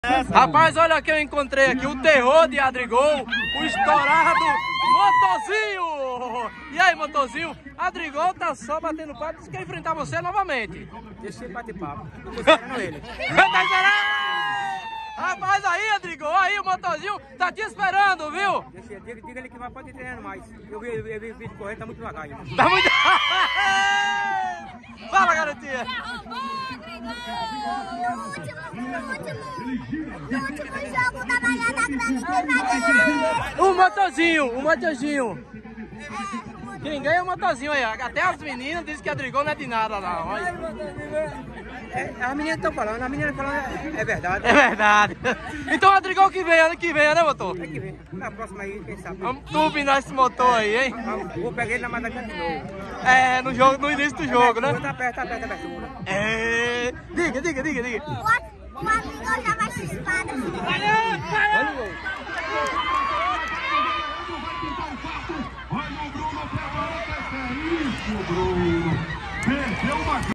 O Motorzinho desafiou Adrigol novamente
O terror de Adrigol , mandou avisar que essas corridas do Adrigol não assusta ele , e desafiou Adrigol a correr na abertura da copa rural 2023 em Malhada Grande .